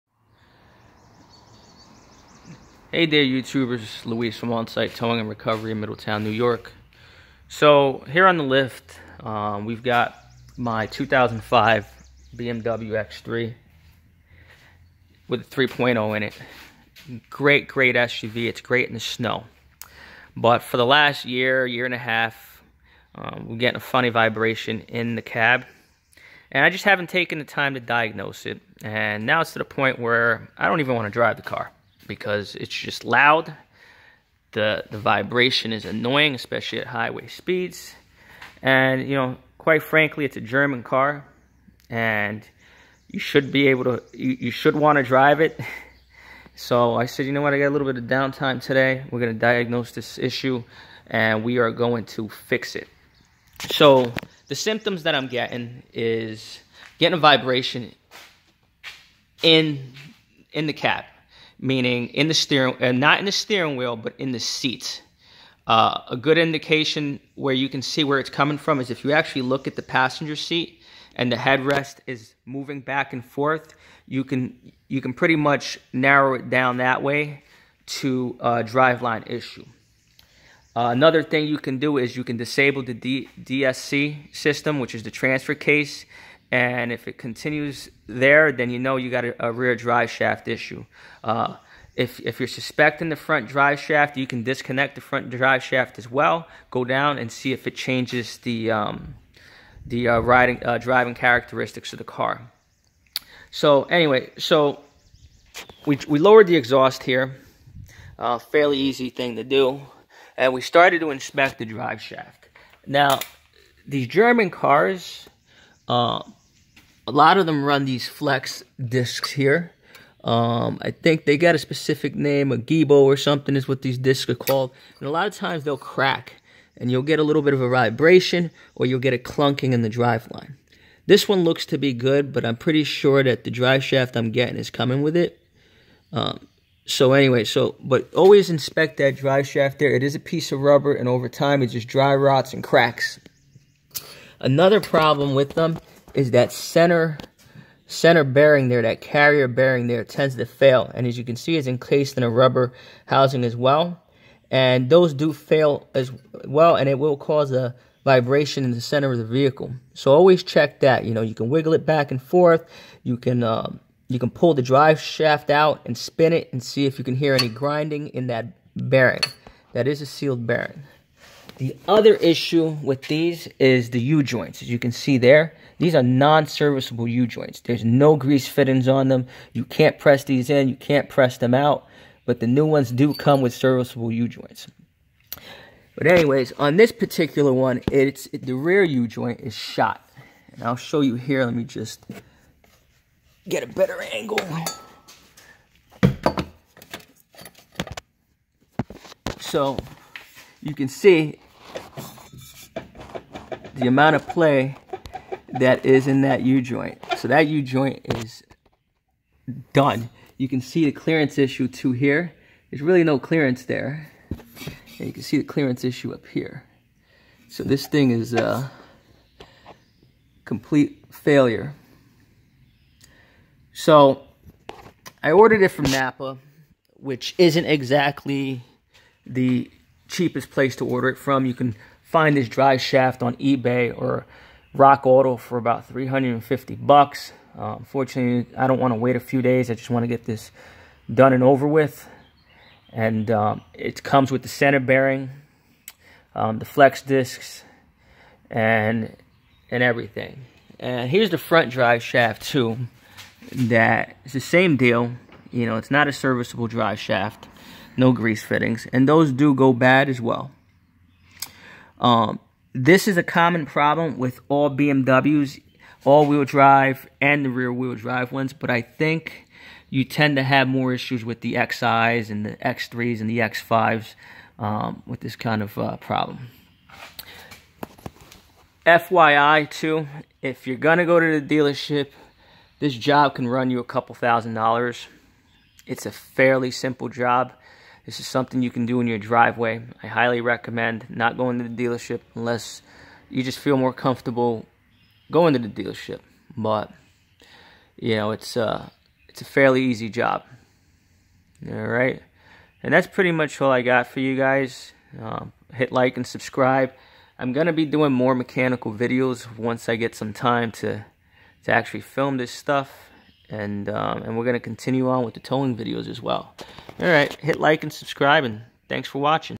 BMW Vibration / Drive shaft Repair
My X3 was suffering from a really bad vibration in the cab. A quick inspection turned out a bad drive shaft. These drive shafts do not have serviceable U Joints. This is a common problem on most XI models.